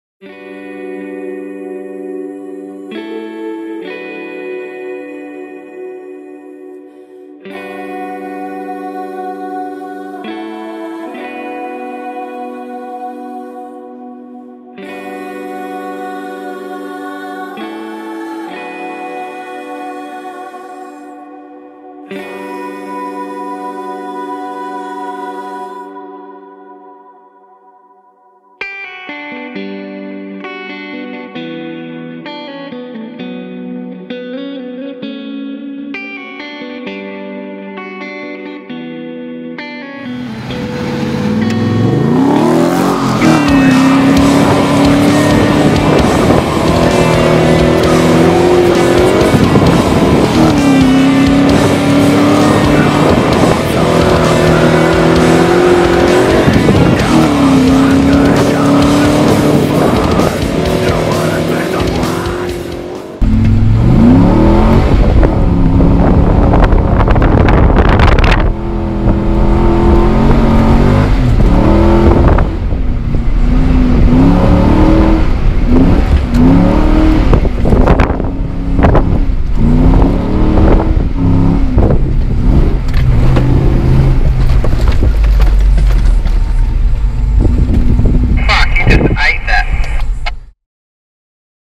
LS GU PATROL LOVES THE SAND!
Driving up the tricky sandy section Ngkala Rocks on Fraser Island... Didnt expect to go this quick!!